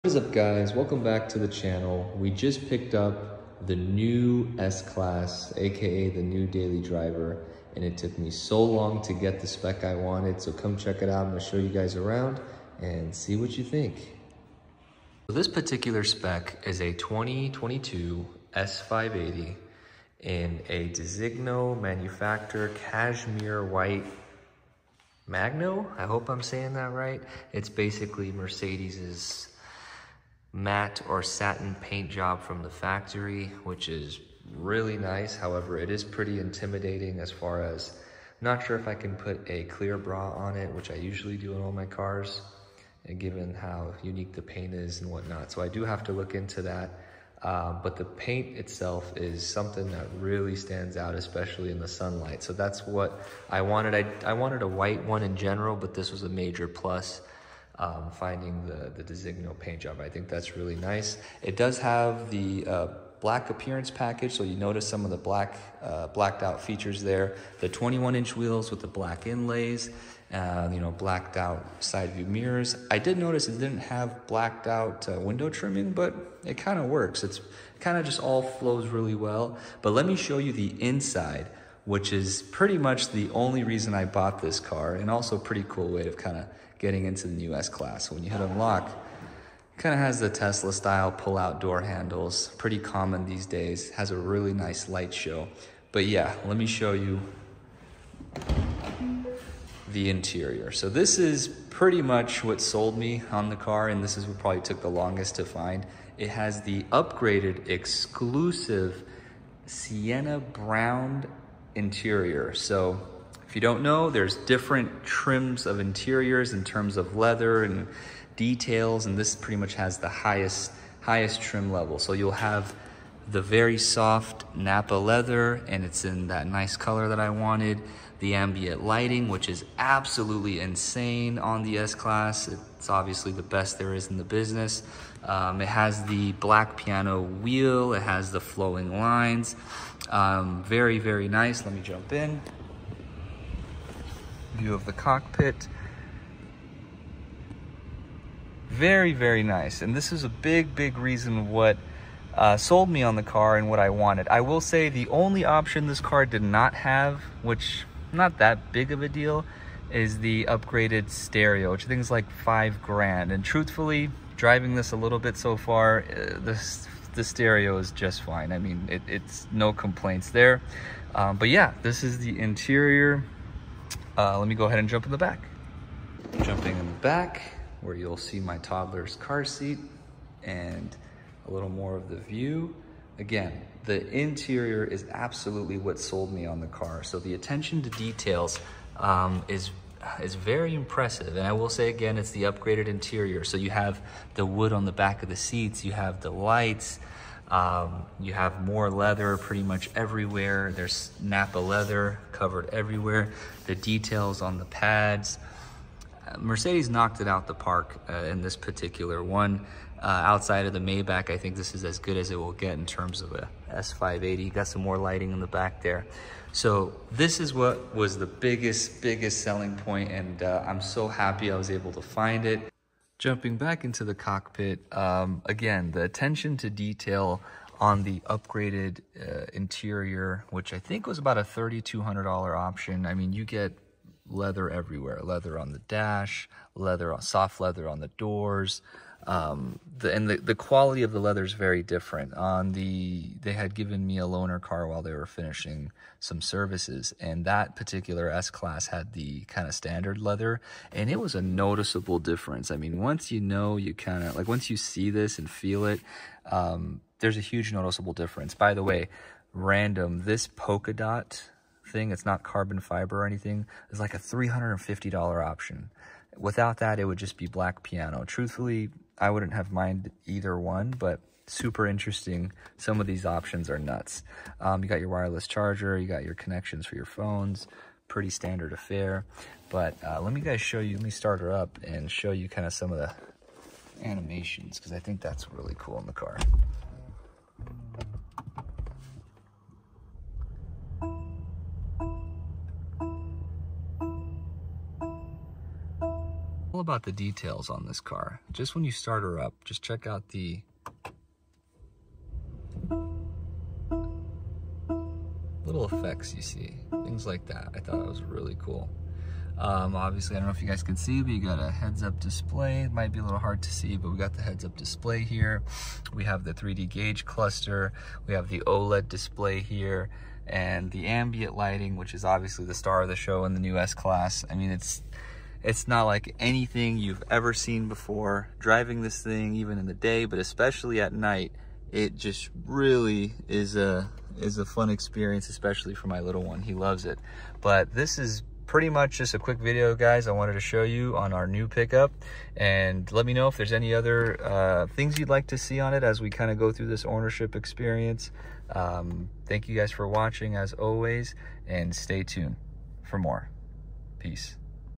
BOUGHT A NEW MERCEDES-BENZ S580: MY SPECIAL ORDER DAILY DRIVER/QUICK TOUR
Just bought a new S-Class in factory matte white paint. Here is a quick tour of it :)